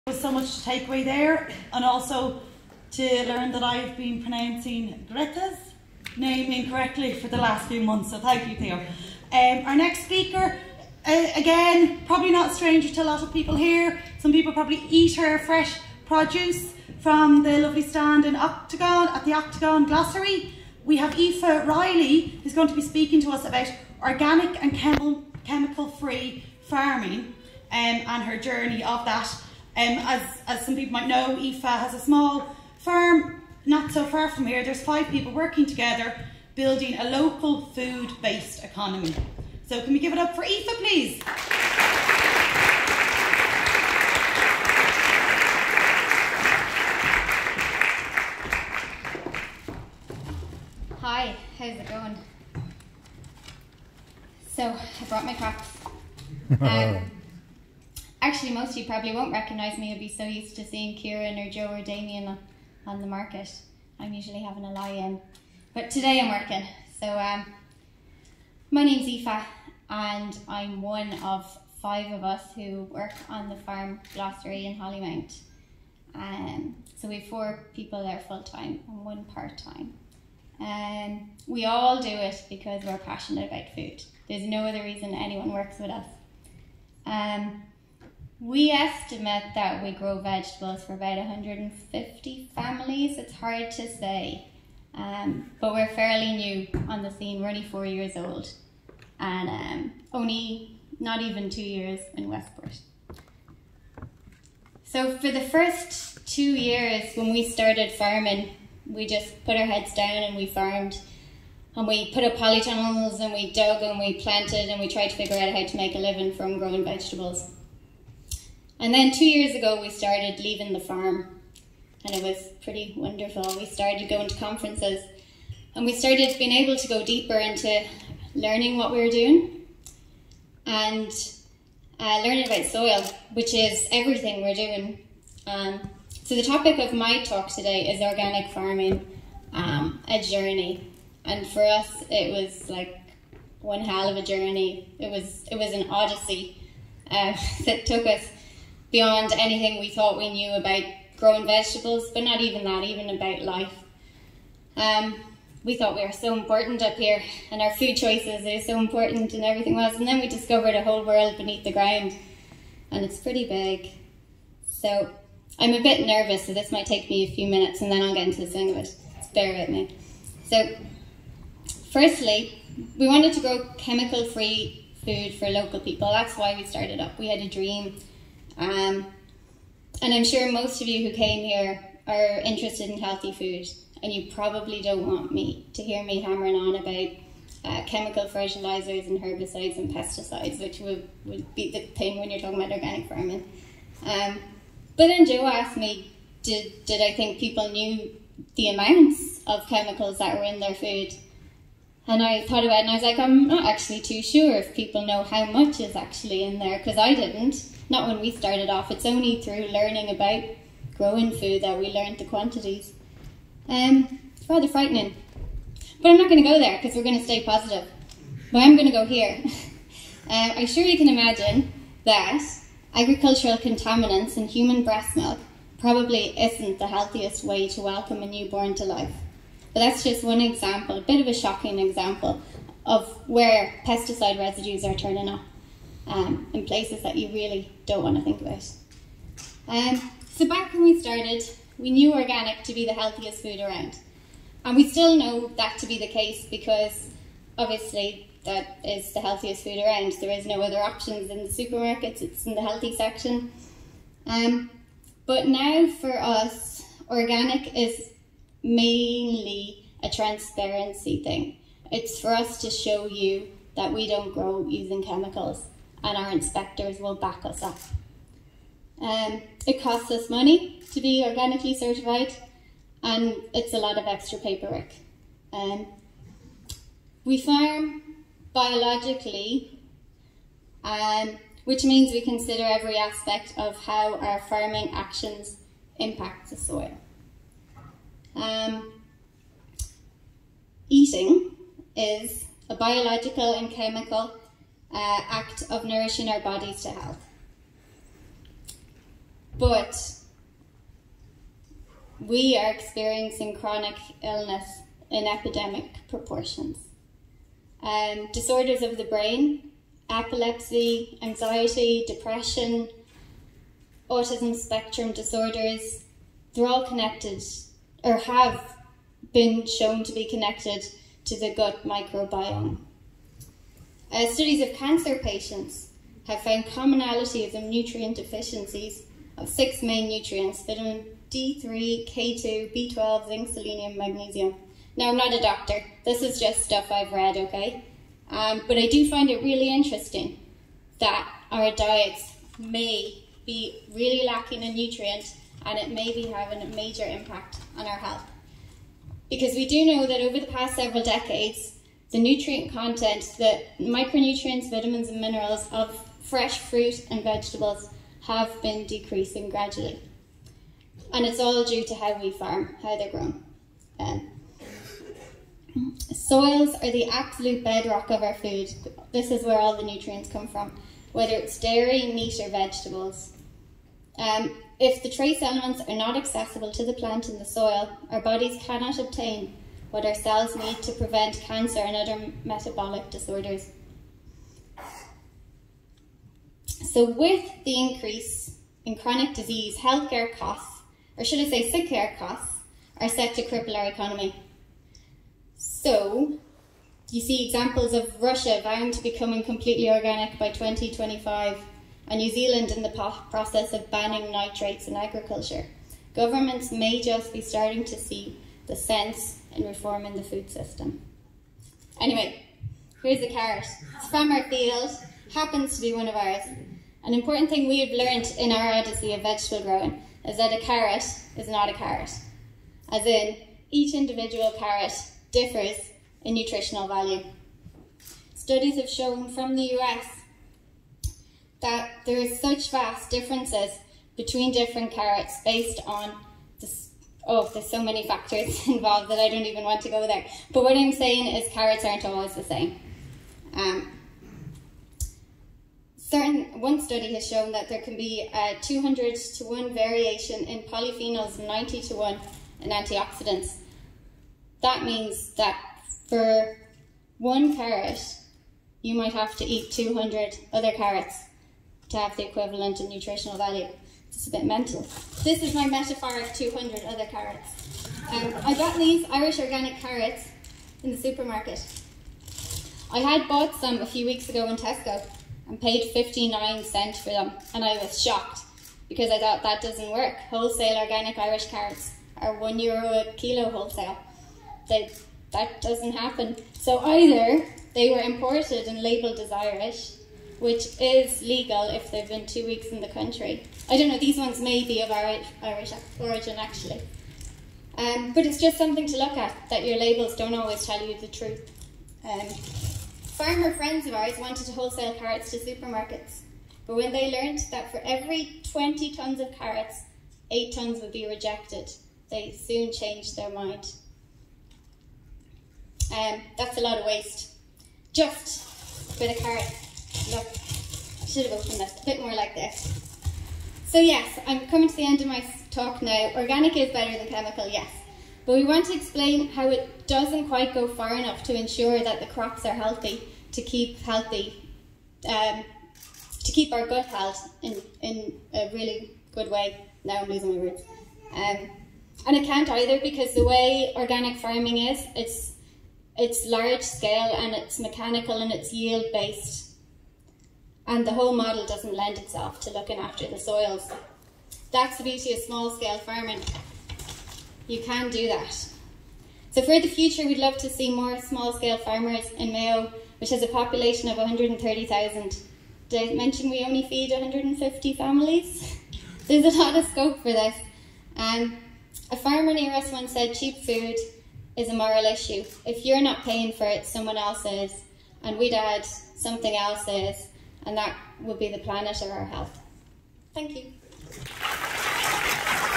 Aoife Reilly: Organic & Chemical Free Farming: 16.11.2019.
Aoife Reilly talks about Organic Farming at the Edible Landscape Project seminar held in Westport Town Hall Theatre. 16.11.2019